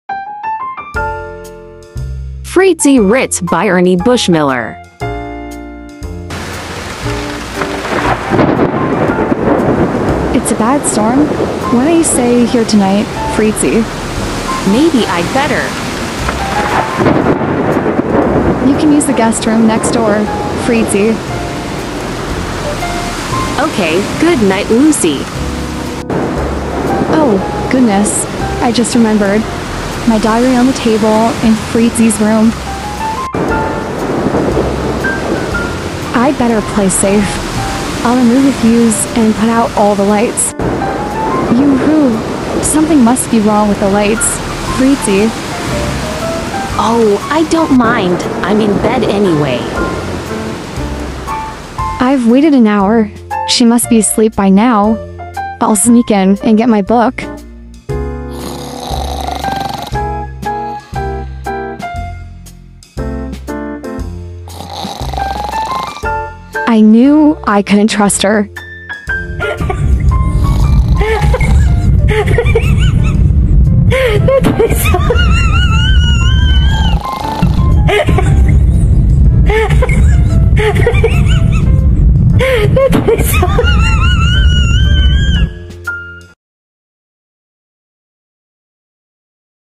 Fritzi Ritz - The Diary - Humor Funny Comic Book Strips Spinoff
Fritzi Ritz" is a comic strip created by Ernie Bushmiller that originally debuted in 1922. The strip primarily revolves around the adventures and misadventures of Fritzi Ritz, a fashionable and independent young woman, along with her friends and family. The storyline often involves humor, romance, and social situations, with Fritzi getting into various comedic scenarios. The strip gained popularity for its lighthearted and relatable content, making it a classic in the world of comic strips.